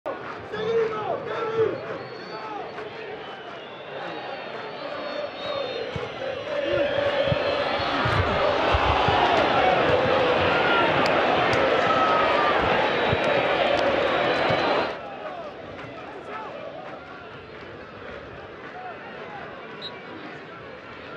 ¡GOL DE RACING! Enzo Copetti | Unión SF vs. Racing (1-1) | Fecha 17 - Torneo de la Liga 2021
Enzo Copetti la rozó tras el centro de Aníbal Moreno y clavó el 1-1 ante Unión.